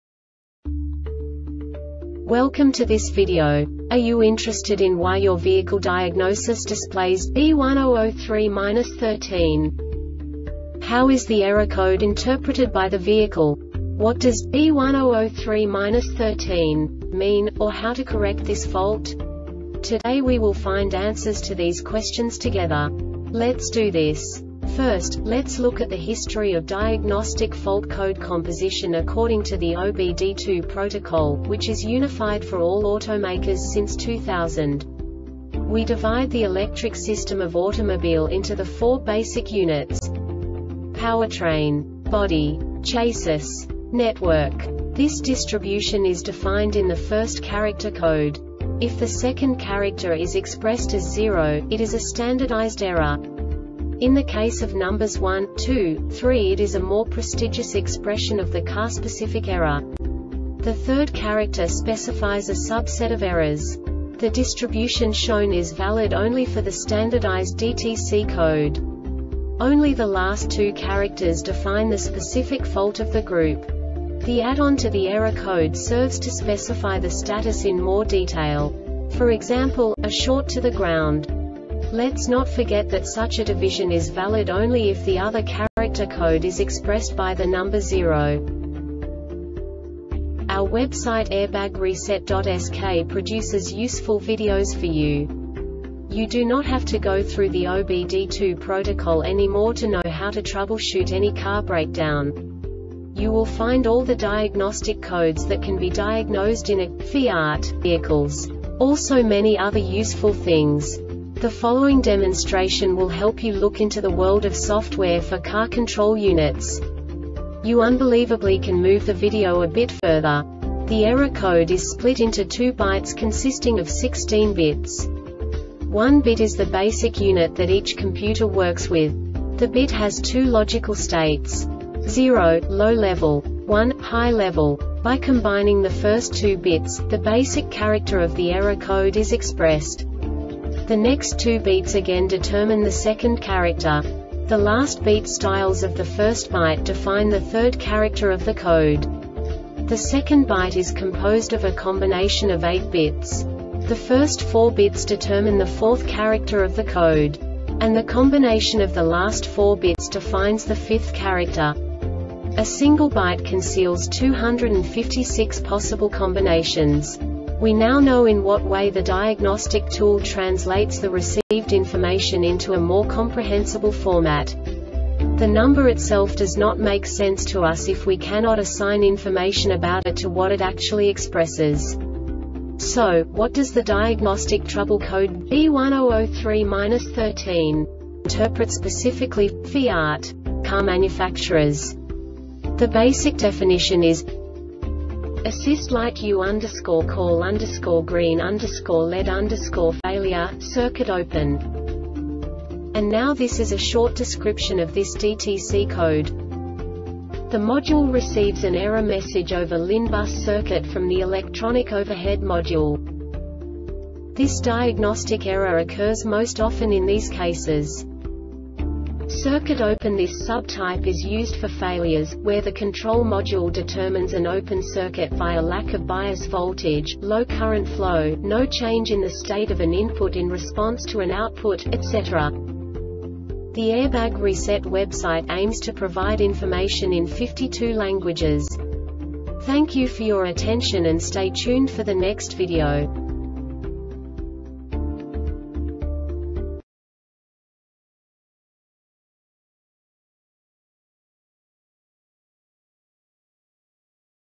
DTC Ford B1003-13 Short Explanation
Contents:
0:21 Basic DTC analysis according to OBD2 protocol standard.
1:48 Insight into programming
2:58 A diagnostically understandable description of the Diagnostic Trouble Code
3:05 Basic error definition

Make:
FIAT
Code:
B1003-13
Definition:
Assist Light U_Call_Green_Led_Failure - Circuit Open
Description:
The module receives an error message over LIN Bus circuit from the Electronic Overhead Module.
Failure Type:
Circuit Open